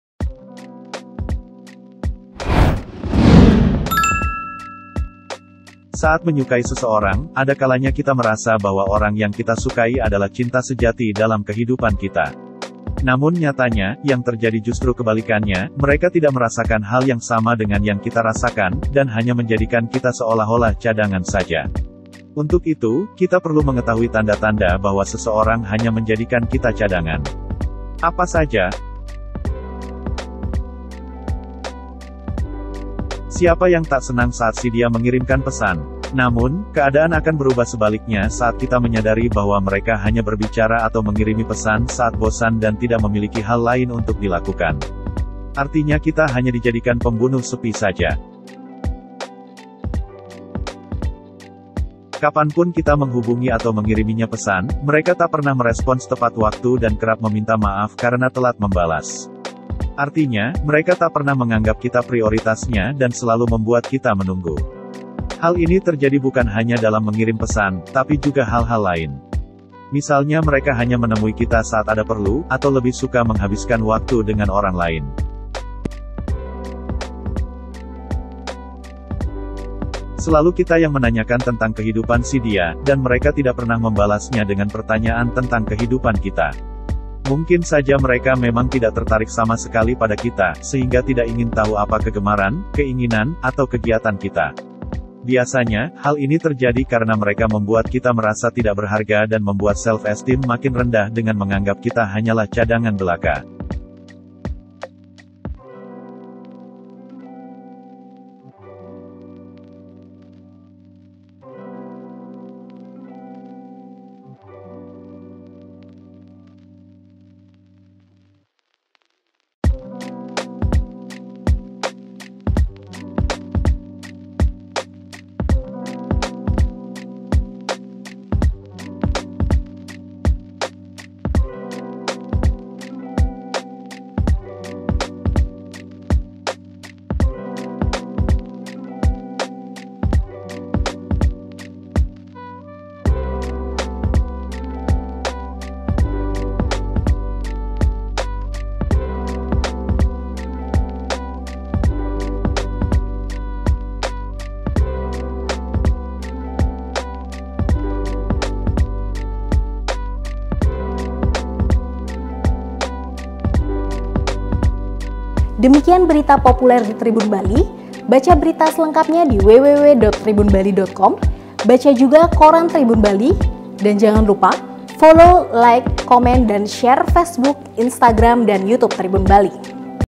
Dia Tak Perhatian Bisa Jadi Kamu Hanya Jadi Kekasih Cadangan, Kenali 4 Tanda Kamu Hanya Cadangan
Saat menyukai seseorang, ada kalanya kita merasa bahwa orang yang kita sukai adalah cinta sejati dalam kehidupan kita.

(paw)